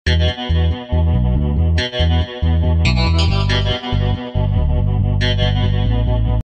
After Effects: Outer Space Vortex 4 !
i used the green egg against the blue sky.. The rotation and the picture is a lot better in this one.